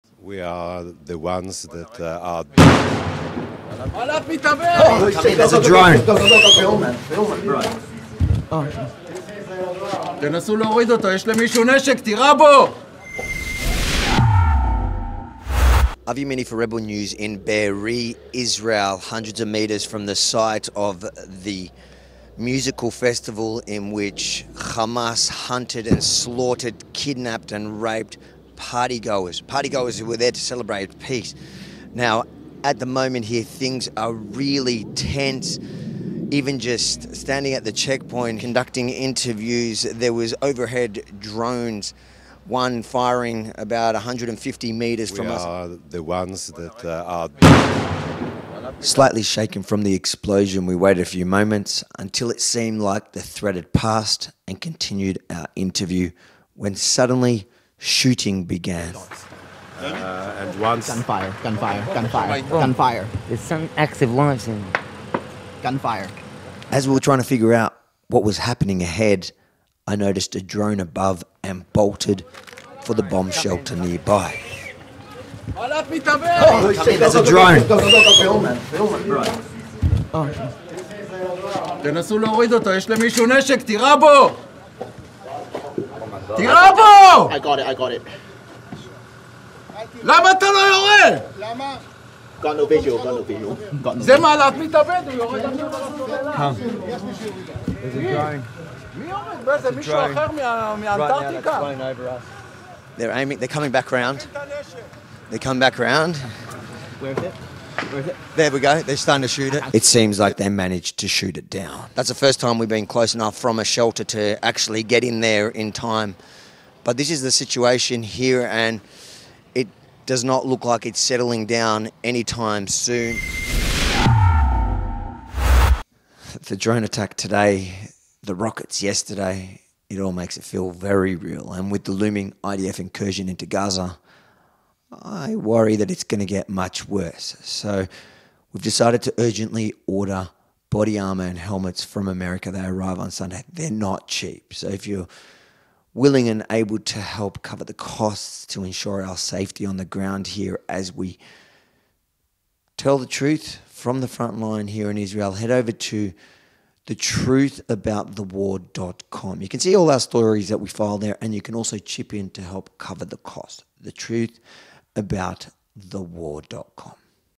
Escaping ATTACK DRONE on the Gaza border
Two days, two close calls—I've ordered body armour.